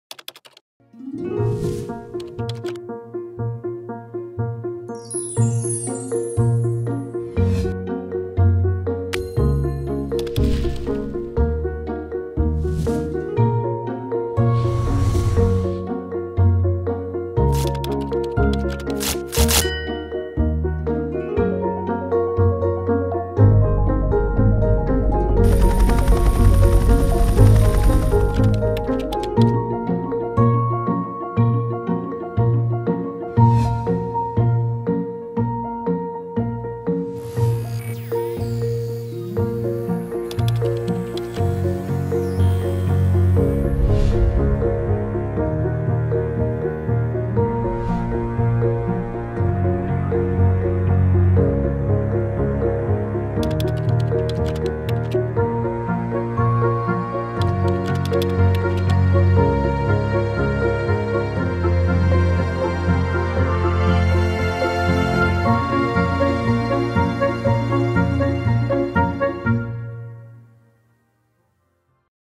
Diputación de Alicante, a un clic de ti
Presentación de la nueva Web de la Diputación Provincial de Alicante "A un click de ti"